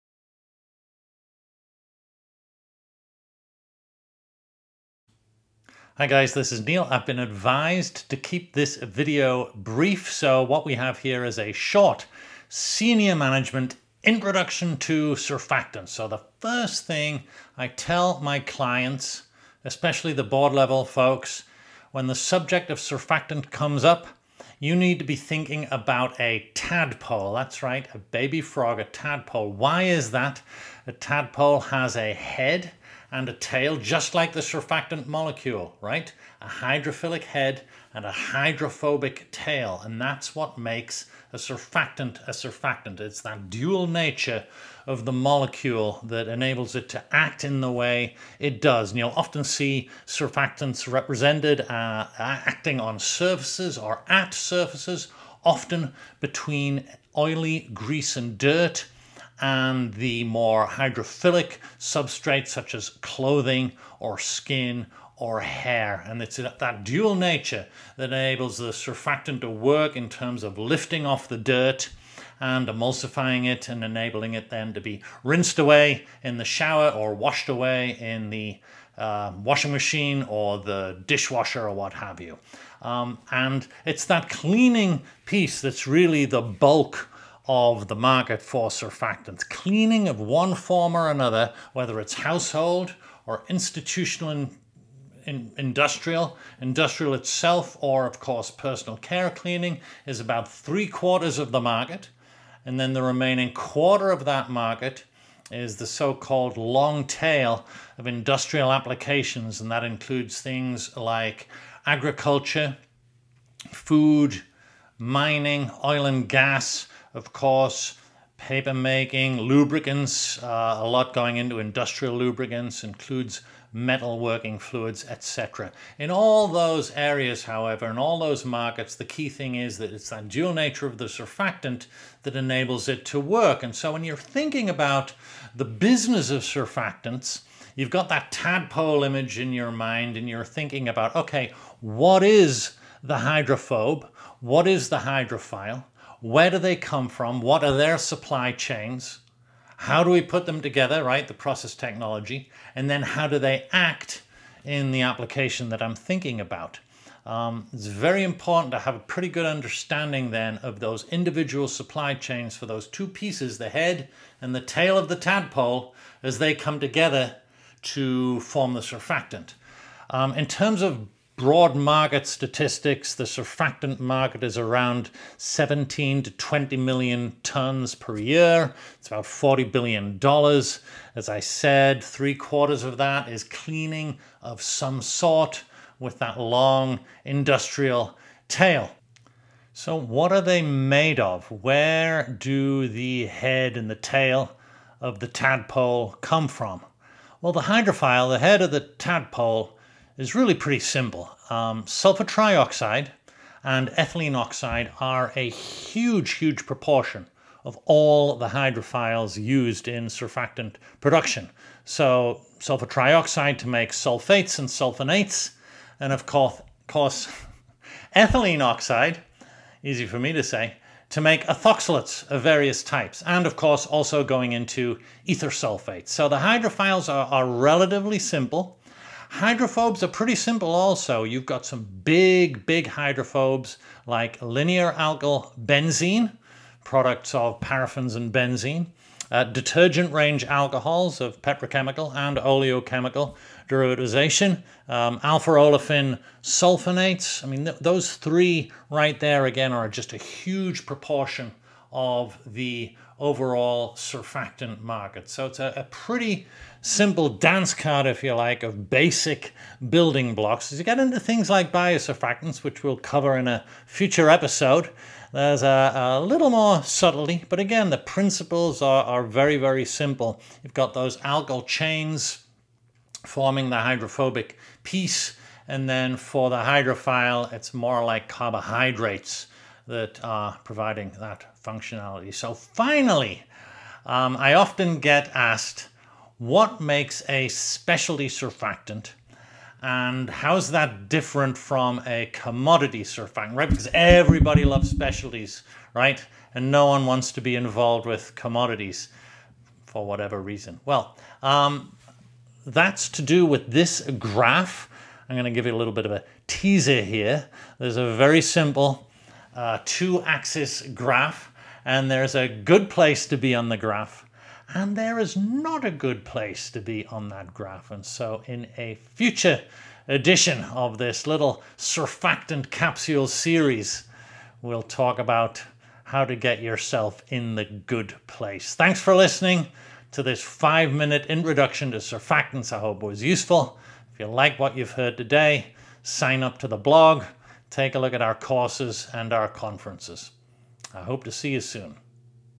A 5 Minute Intro to Surfactants
Links to resources below:  ⟱⟱⟱⟱⟱⟱⟱⟱⟱⟱⟱⟱⟱⟱⟱⟱⟱⟱⟱⟱

In this high level introduction to surfactants, we'll go through what they are, what they do and where they come from. You'll learn about the tadpole framework to understand the market and it's size and shape. Surfactants are used in cleaning of all types - household, industrial and institutional and of course, personal care. There is also a long tail of industrial applications in areas like agrochemicals, metalworking, paper, oil and gas, mining, food and many more. Major players include Stepan, Dow, Syensqo, Indorama, Pilot, Kao, P&G and many others. This is taste of what you can get at our conferences and courses. So if you like what you hear in this intro then consider the following:

Come to the conference May 9 - 10th (2024) Jersey City NJ